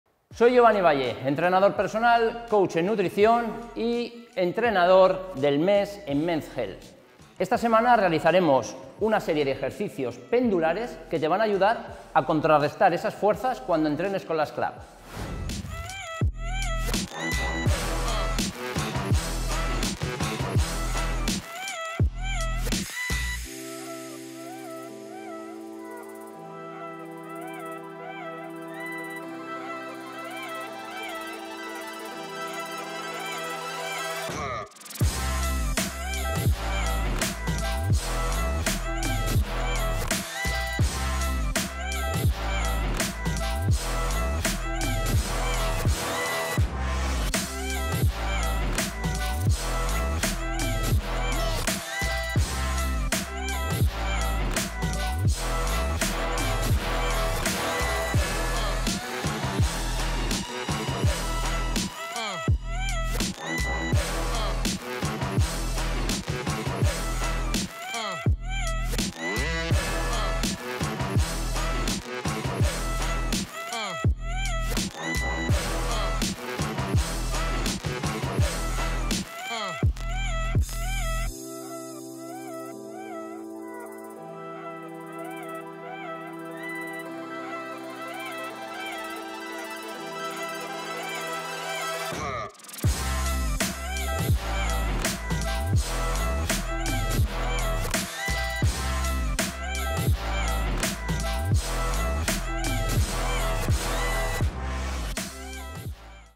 Clubbells: ejercicios pendulares y control de fuerzas con Giovanni Valle | Men's Health España
El entrenador del mes de Men's Health en abril, Giovanni Valle, nos propone un entrenamiento con clubbells. Para esta tercera semana toca realizar ejercicios pendulares y control de fuerzas.

Texto: Roberto Cabezas - @robberpau
Vídeo: Luis Cárcamo
Foto: Ana Ruiz - @anitart
Grooming: Tatí Garú - @tatigaru.makeup
Total look: Hoka y New Balance